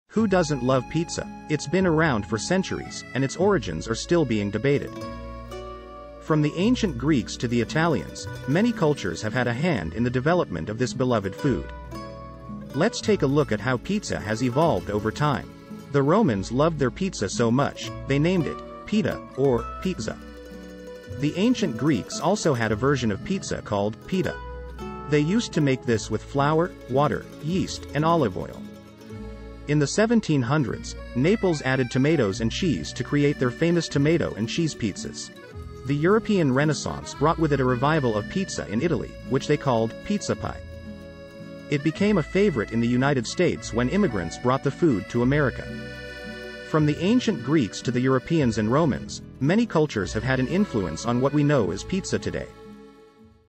Where Did Pizza Come From? | History Of Pizza
Where Did Pizza Come From? The history of the pizza is very fascinating. In this video, we're going over some facts about where pizza came from.